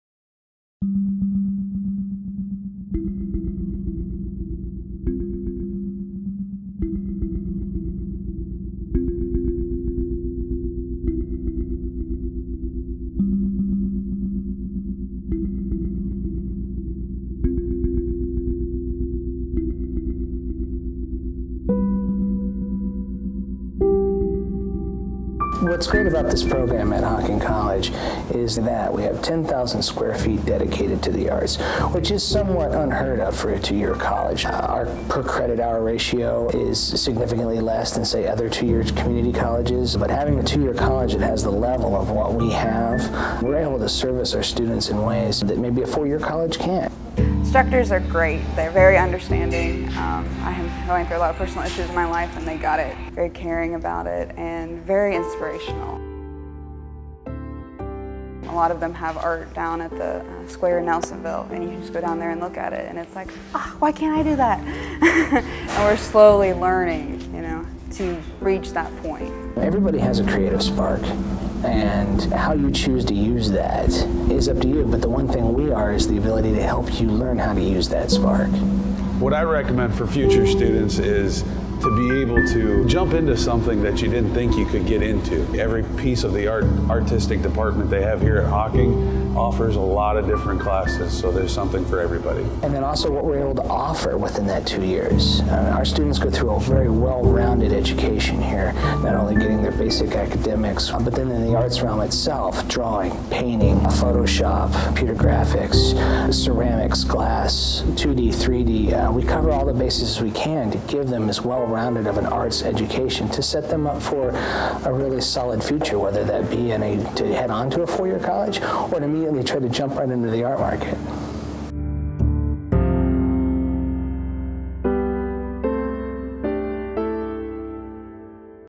Art, Design and Marketing Promotional Video for Hocking College
A promotional video i did for Hocking College's art, design and marketing program.